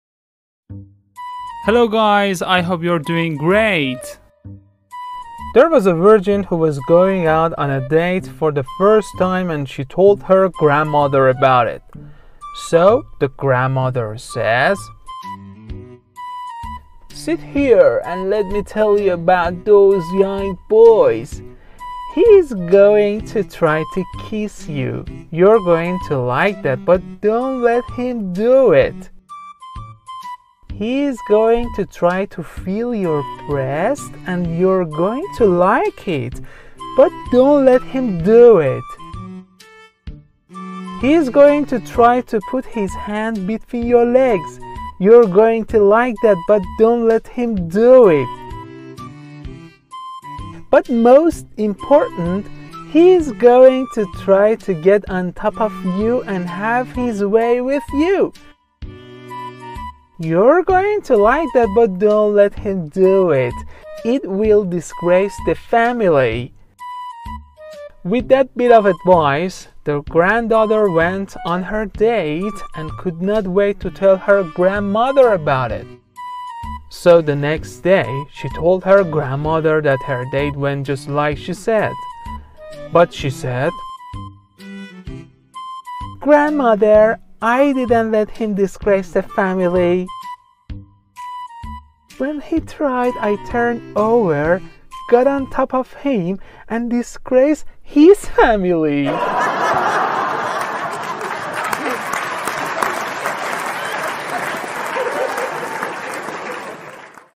😂 Best Jokes of the Day | Dirty Jokes | Funny Jokes
This is our 102nd Funny Jokes. In this dirty joke , a girl dated a man and she wanted to take off her. 😁🤣

we collect lots of jokes such as dirty jokes , Dad jokes , Funny Jokes , bar Jokes and. 😊😁